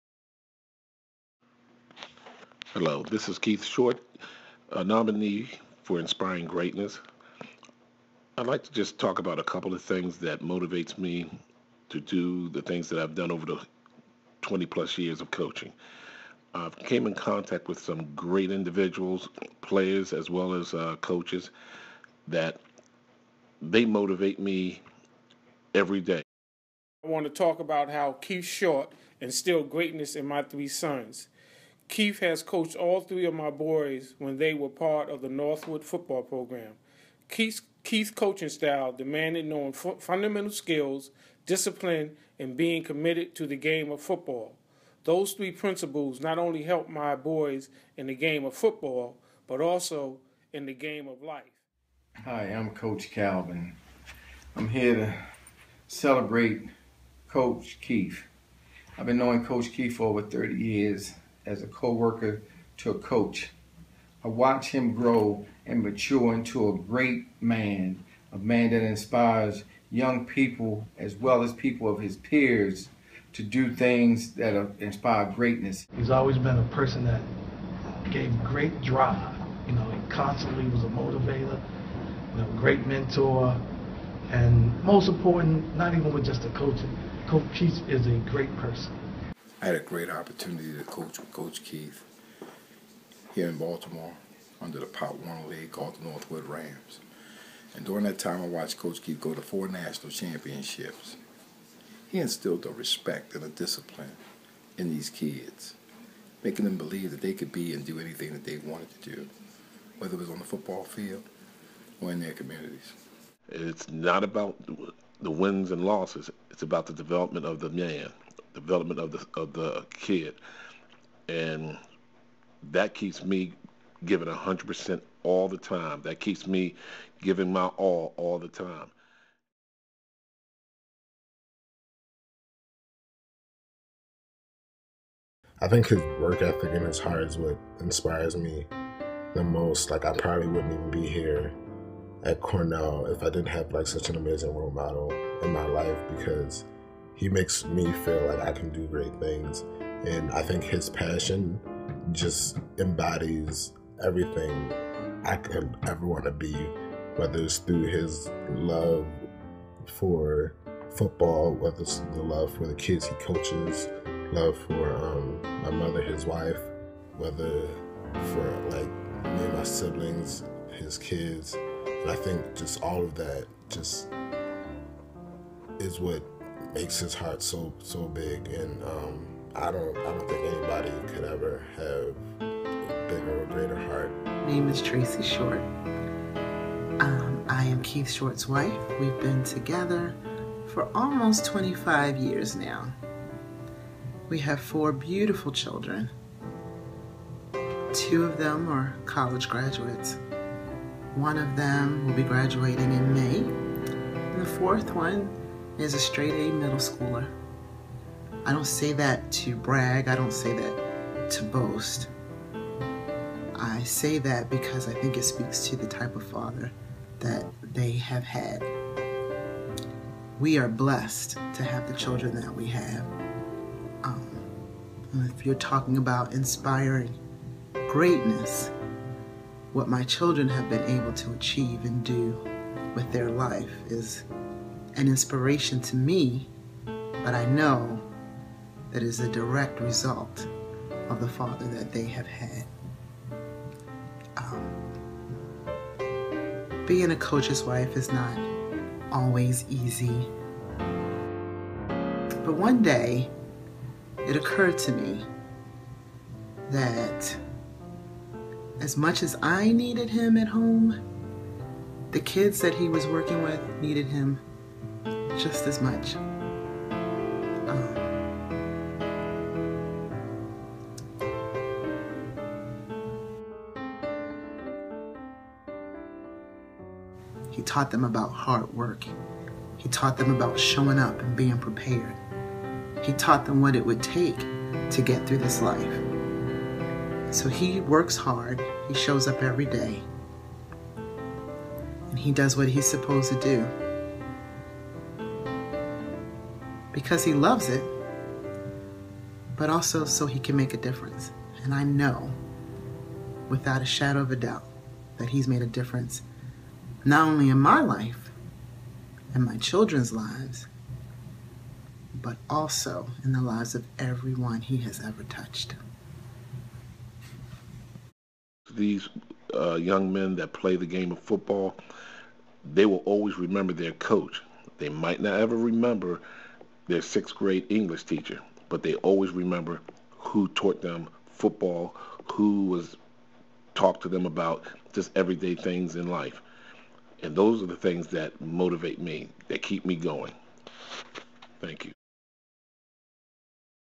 Inspiring Greatness, Keith Short
Follow Keith Short's journey of inspiration and dedication.  Journey through Keith's professional coaching career to see how he has inspired young athletes to be strong men.